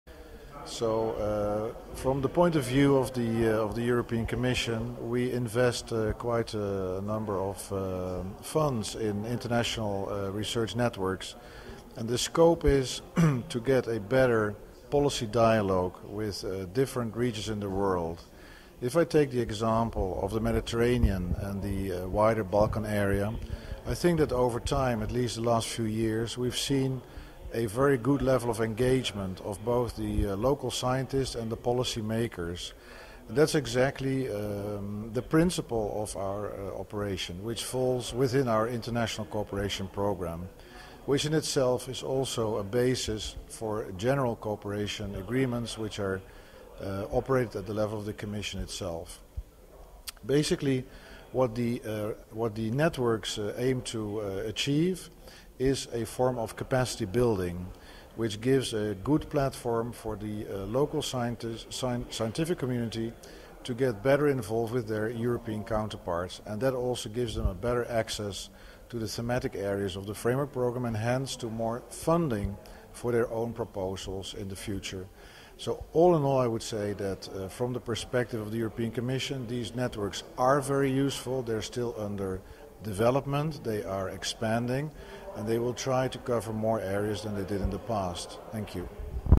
WSD2010-Dr Robert Burmanjer, Head Unit D-3, Specific Int. Cooperation Actions, Eu Commission
WSD2010 Venice - Interview with Dr Robert Burmanjer, Head of Unit D-3: Specific International Cooperation Actions, European Commission-
Question : Are international networks making a difference in science? Dr Burmanjer replies that "the European Commission invests quite a number of funds in international research networks. The scope is to get better policy dialogue with different regions in the world. As to the Mediterranean and wider Balkan areas, over time - at least in the last few years - we have seen a very good level of engagement of both, local scientists and policy makers."